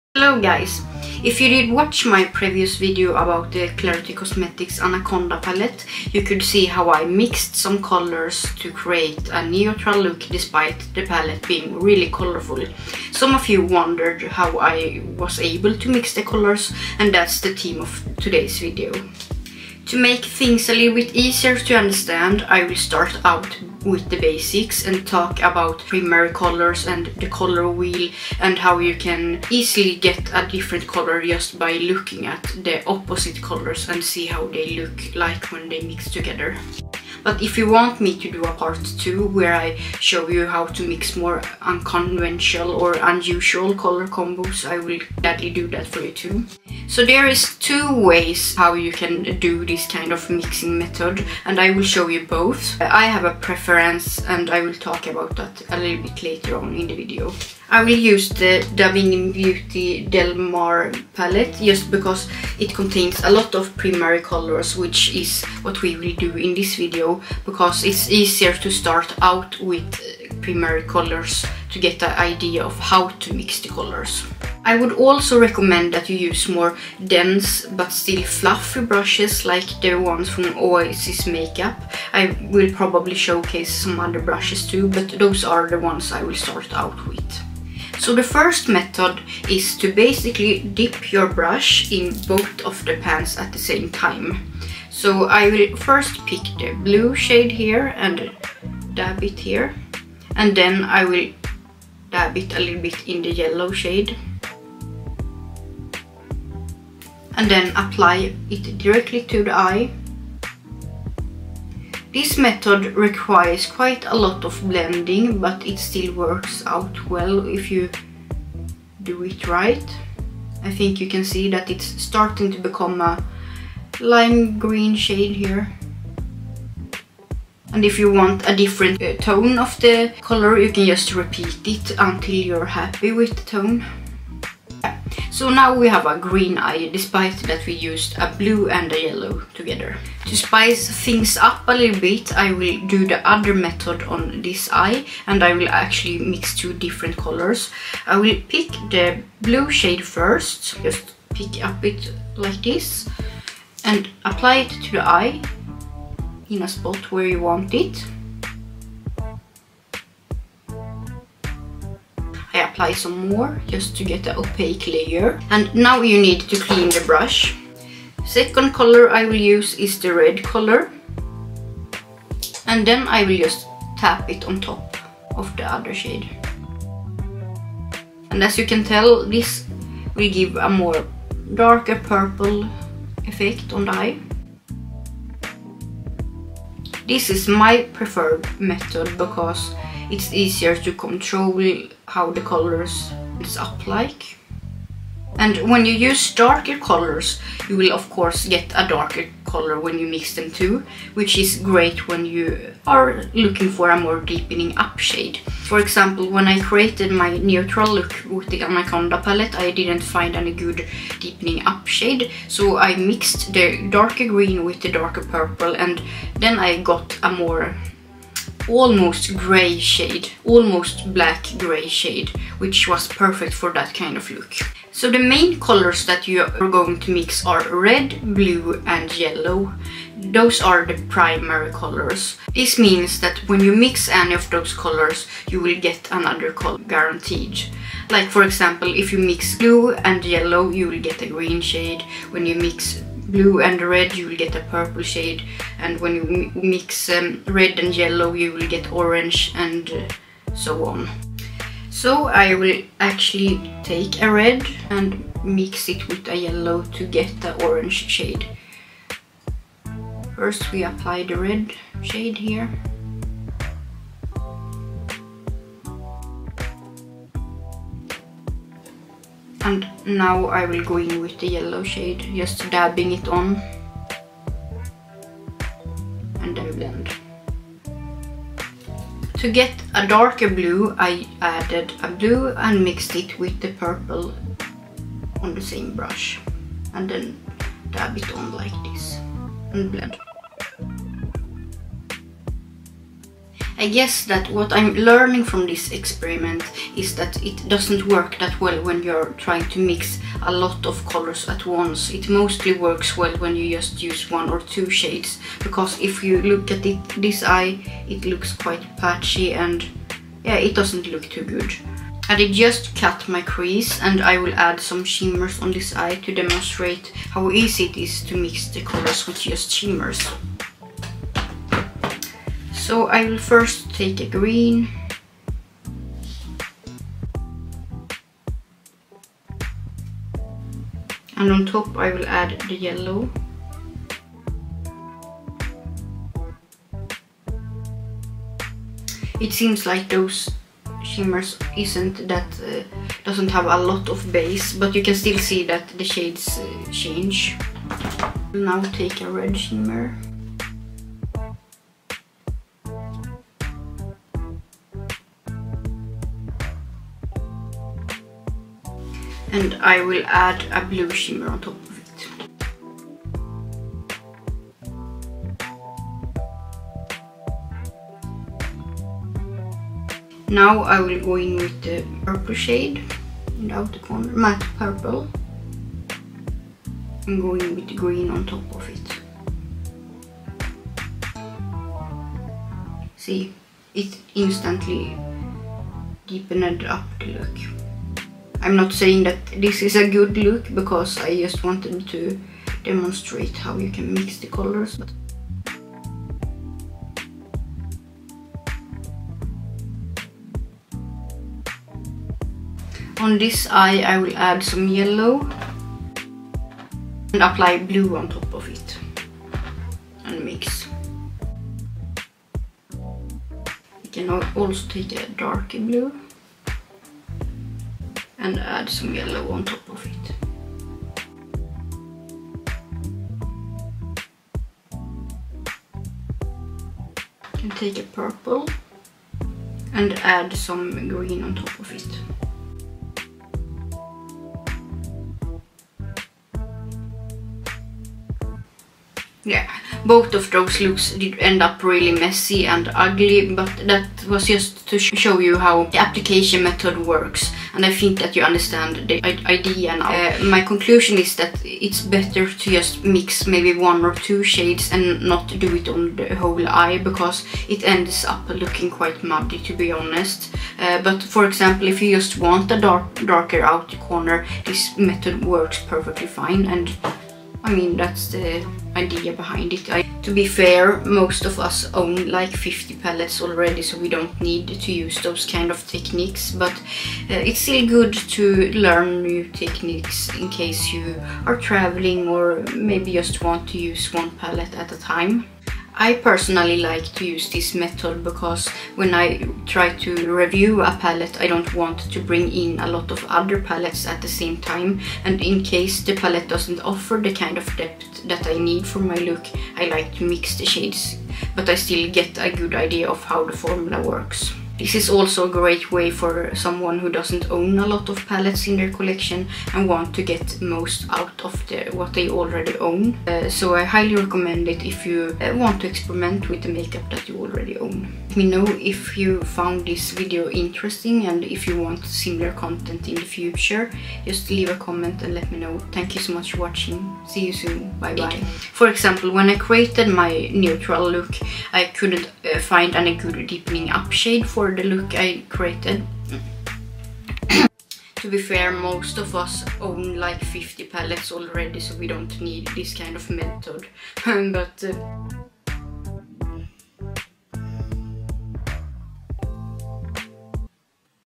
HOW TO: MIX EYESHADOW COLOURS - THE BASICS | FAIL EDITION - EVERYTHING WENT WRONG
Hello friends!

After filming this video I realized that I'm not the best teacher and I also realized that the methods I used isn't as fool proof as I first thought. But hopefully you will still learn something from this video - and if not, I hope that you will at least get a good laugh at all my mistakes!